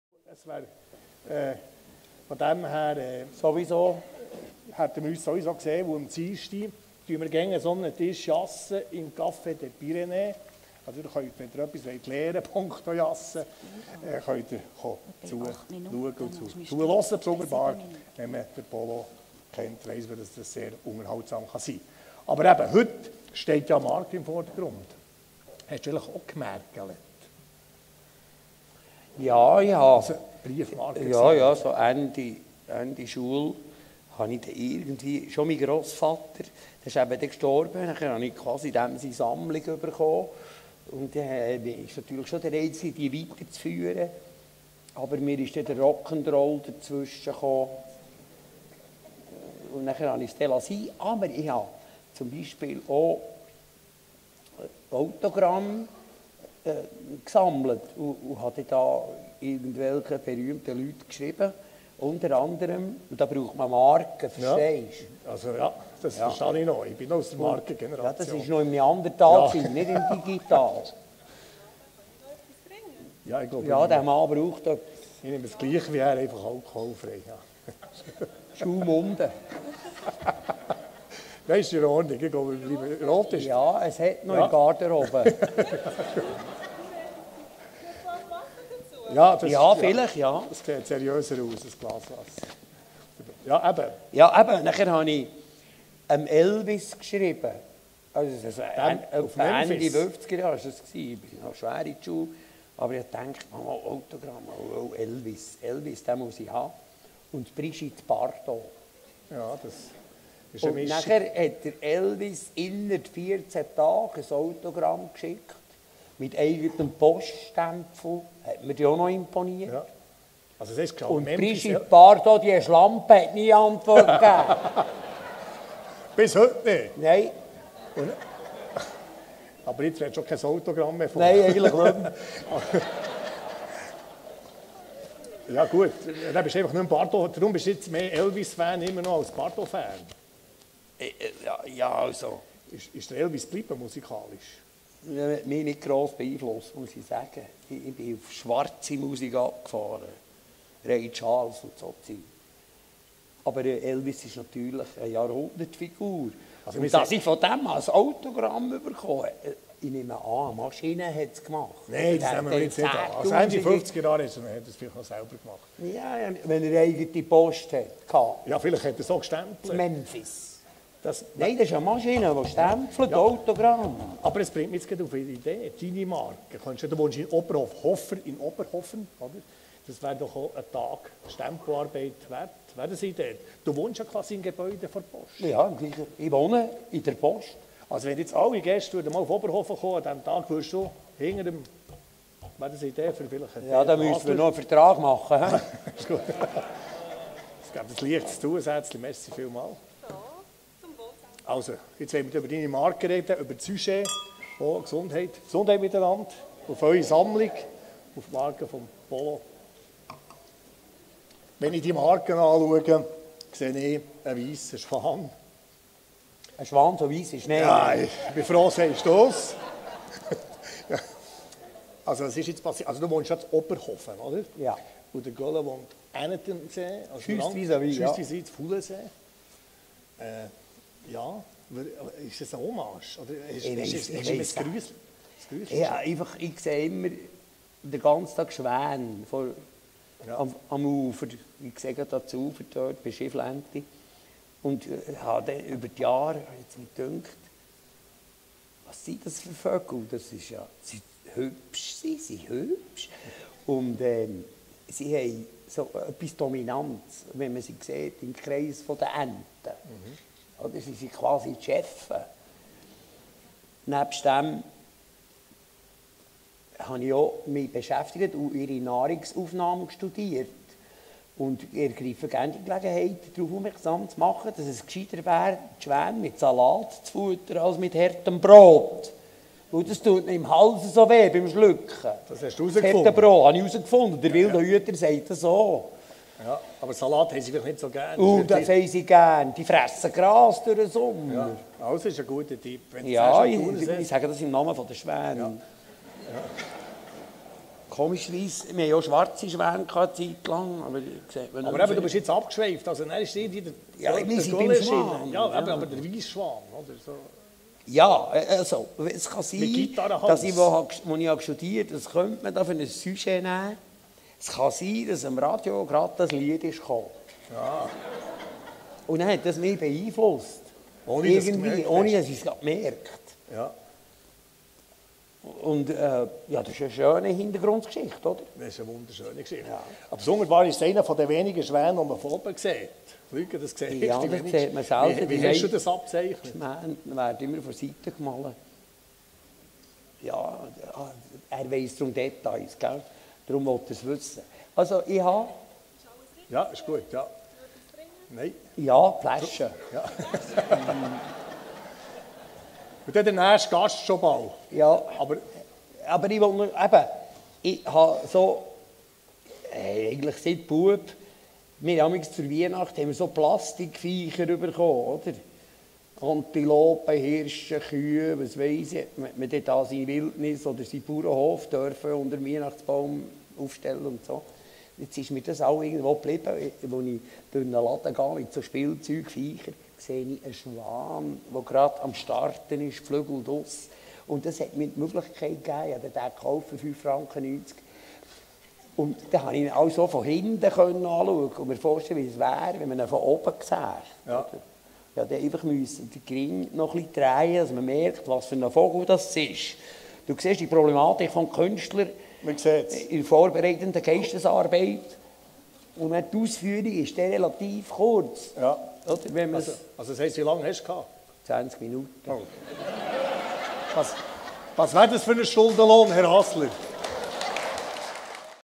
Polo Hofer und Matthias Mast
Die Schweizerische Post gibt jedes Jahr einer prominenten Schweizer Persönlichkeit die Möglichkeit, eine Briefmarke zu gestalten. 2013 war Polo Hofer der Auserwählte.
Die Sondermarke im Wert von einem Franken wurde am 5. November feierlich lanciert.